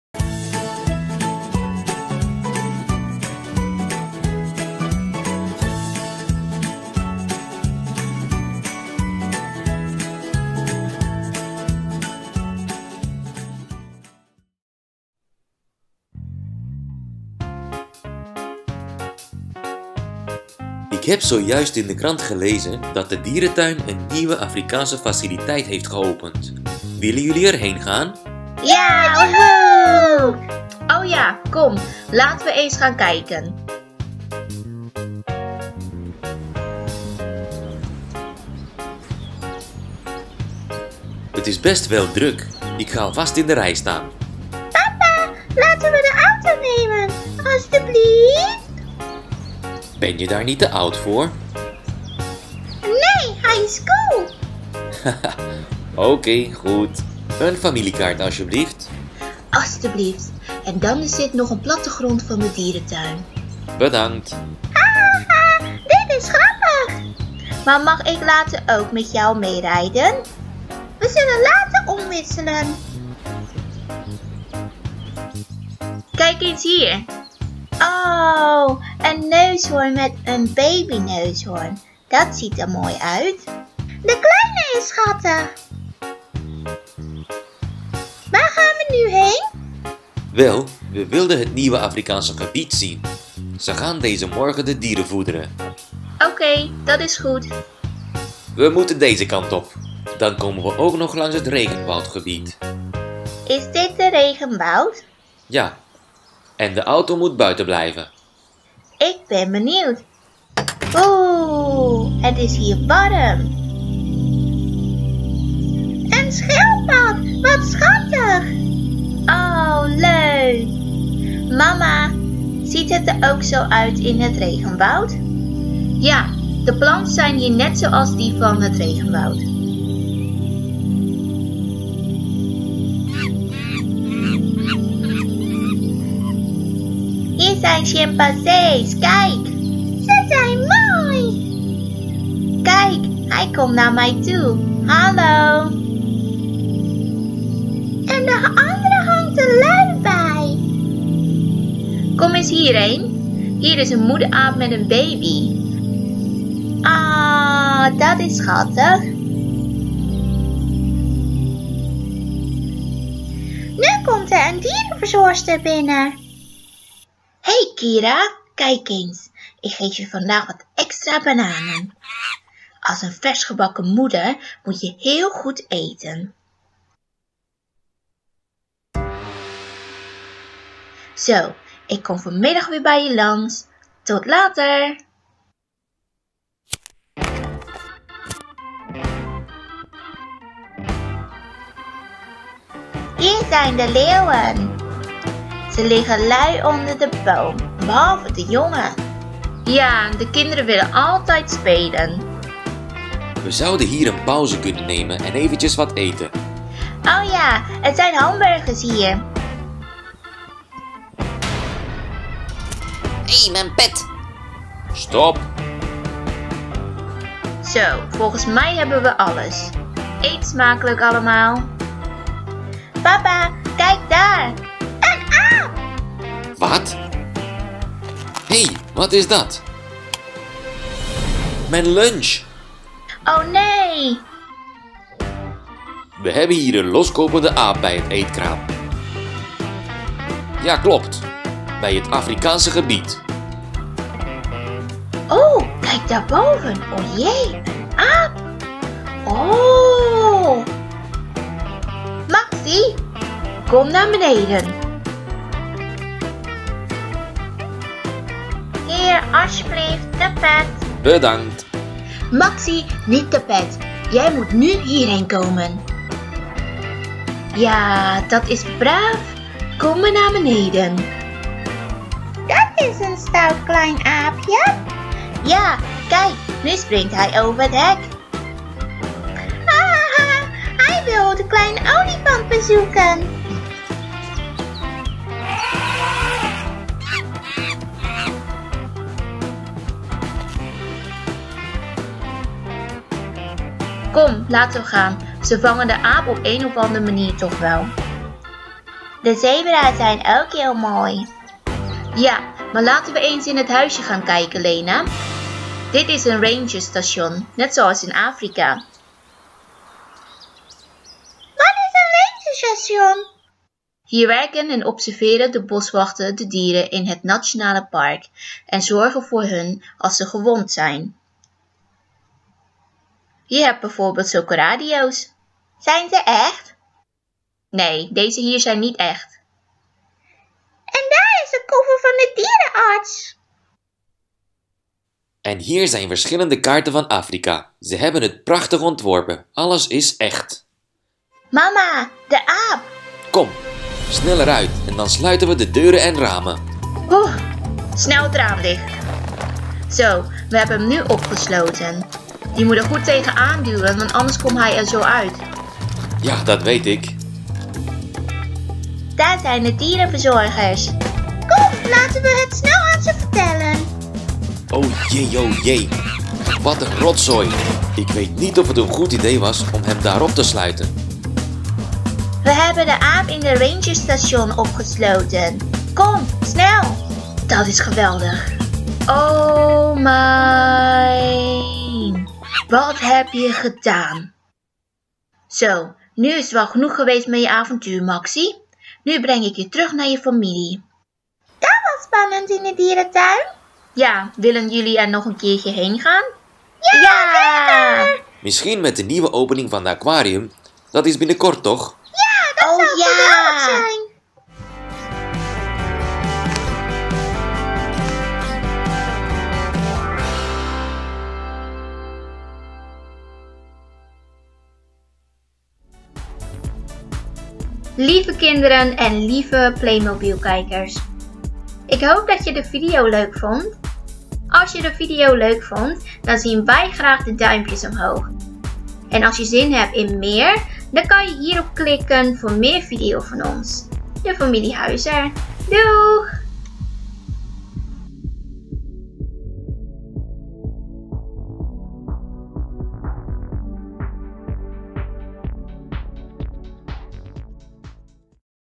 Playmobil filmpje Nederlands De dierentuin - Familie Huizer
Famlie Huizer bezoekt vandaag de Playmobile-dierentuin. Er is daar een nieuwe Afrikaanse gebied geopend met olifanten, zebra’s en giraffen. En een kleine uitbijter is ook onderweg…
Veel plezier met onze Playmobile-verhaal.